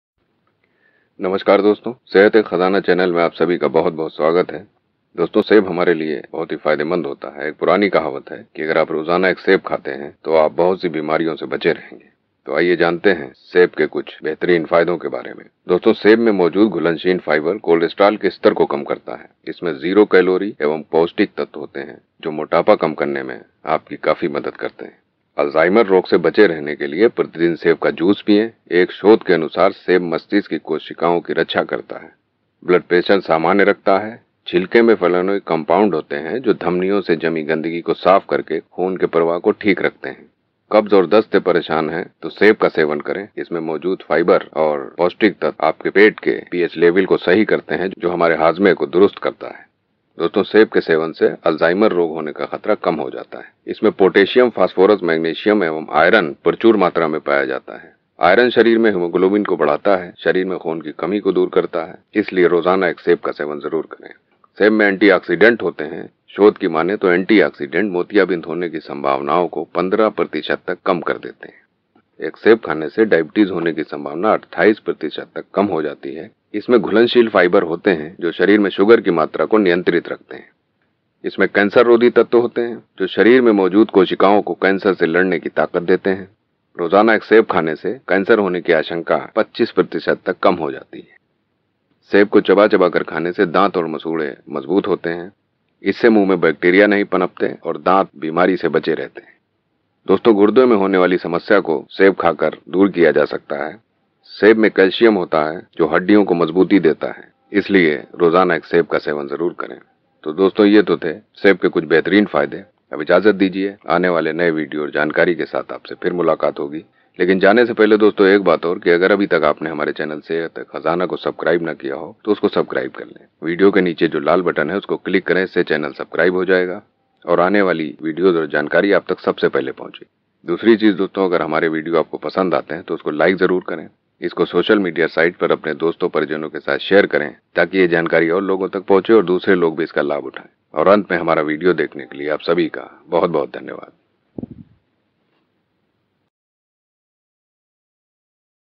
सेब खाने वाले इस विडिओ को ज़रूर देखें | Apple Health Benefits | Hindi
सेब Apple खाने वाले इस विडिओ को ज़रूर देखें | सेब एक फल है सेब का रंग लाल या हरा होता है | वैज्ञानिक भाषा में इसे malus pamila भी कहते है आइये जानते है सेब के कुछ बेहतरीन फायदे amazing health benefits of apples. सेब के इन फायदों के बारे में जानकर हैरान हो जायेंगे आप | किसी ने कहा है health is also wealth.